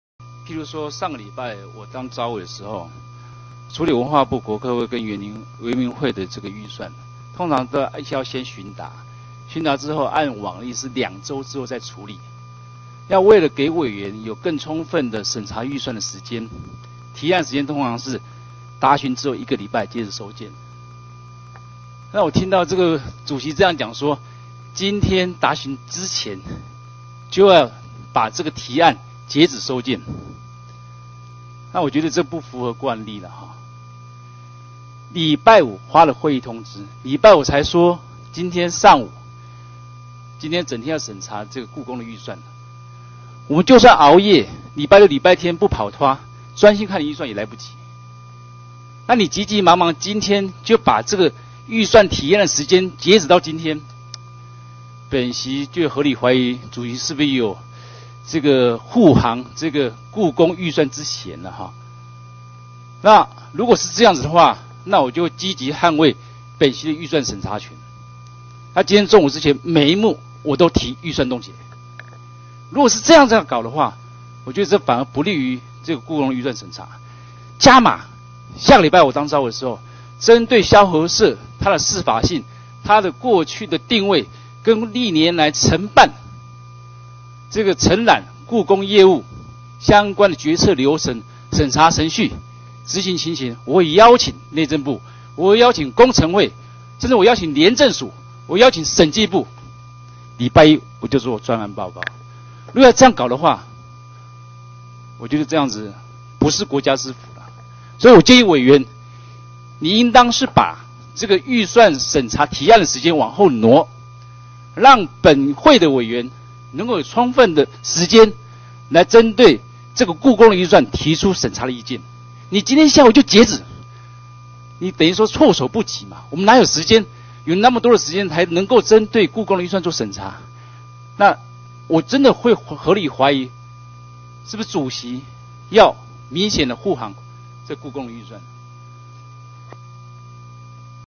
2013-11-04 邱志偉 發言片段, 第8屆第4會期教育及文化委員會第12次全體委員會
會議簡介: 立法院第8屆第4會期教育及文化委員會第12次全體委員會議審查103年度中央政府總預算案關於國立故宮博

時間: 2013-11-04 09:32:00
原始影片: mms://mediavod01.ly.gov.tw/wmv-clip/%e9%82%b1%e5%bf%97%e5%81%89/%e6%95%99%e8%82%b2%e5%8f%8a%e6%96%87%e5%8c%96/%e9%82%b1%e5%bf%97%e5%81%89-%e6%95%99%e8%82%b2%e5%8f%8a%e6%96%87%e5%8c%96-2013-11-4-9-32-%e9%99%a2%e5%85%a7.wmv